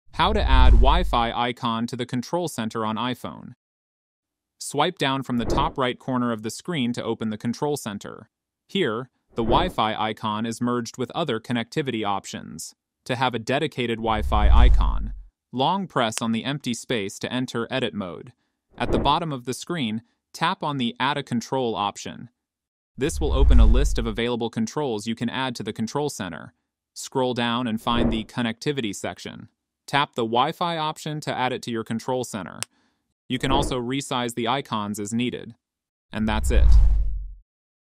How To Add Wi Fi To Control Center on iPhone?
Learn how to get Wi-Fi controls in iPhone Control Center with this quick and easy tutorial! Having Wi-Fi controls in your iPhone's Control Center can be a game-changer, allowing you to quickly toggle your Wi-Fi on and off, change networks, and more. In this video, we'll show you how to add Wi-Fi controls to your iPhone's Control Center, giving you faster access to your Wi-Fi settings. Whether you're trying to conserve battery life or simply want to streamline your iPhone experience, this tutorial is for you!